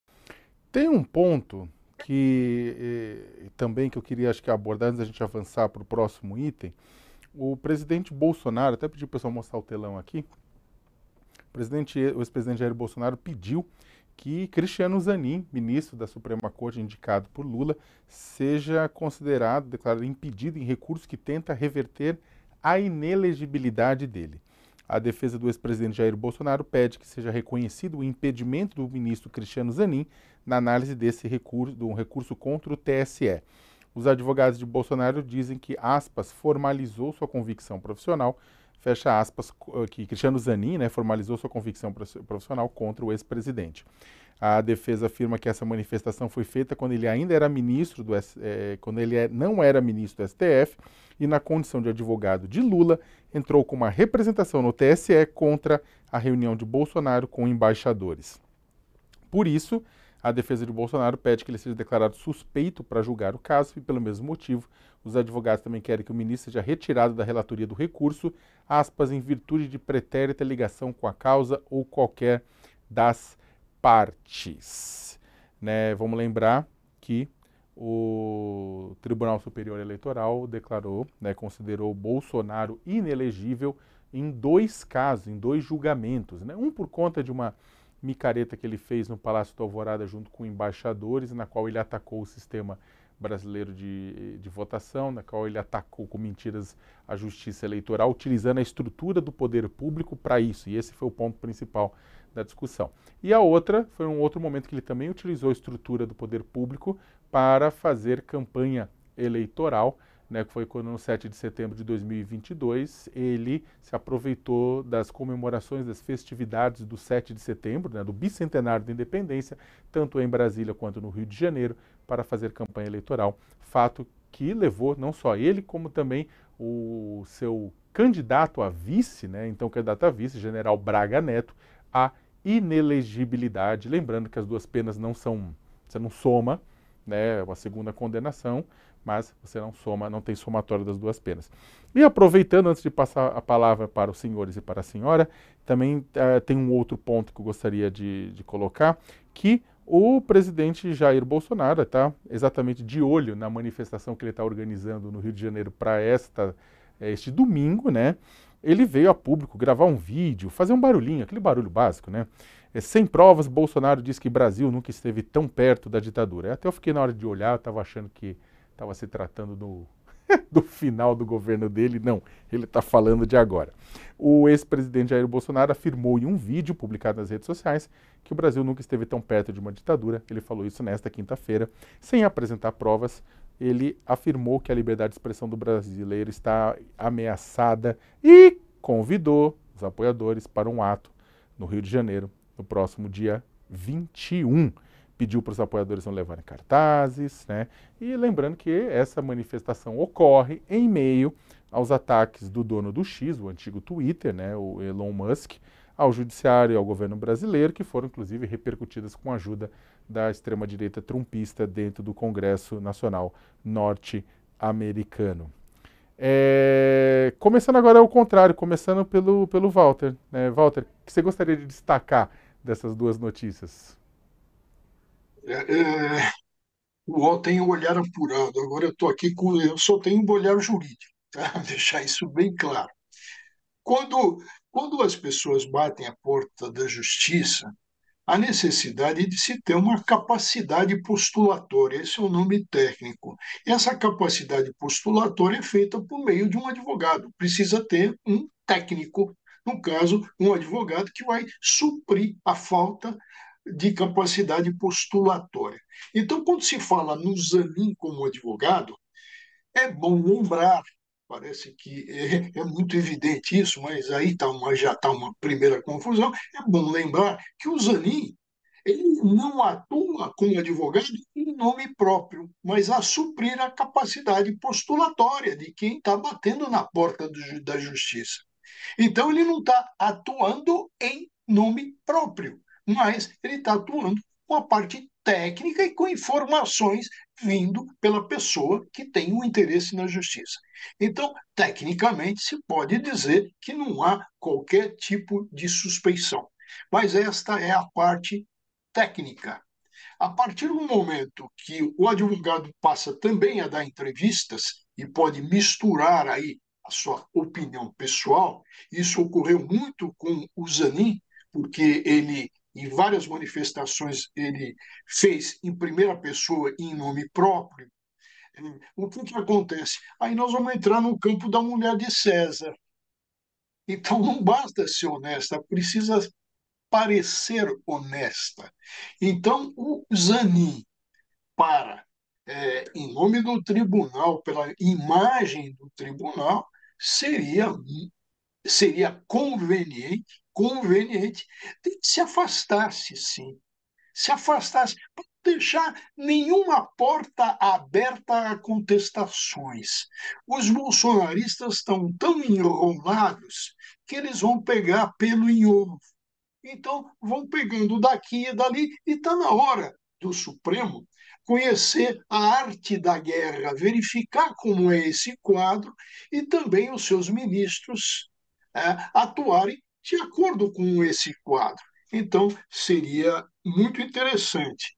Bolsonaro pede Zanin impedido em recurso que tenta reverter inelegibilidade
A defesa do ex-presidente Jair Bolsonaro (PL) pede que seja reconhecido o impedimento do ministro Cristiano Zanin na análise de um recurso contra o TSE que tenta reverter a inelegibilidade dele.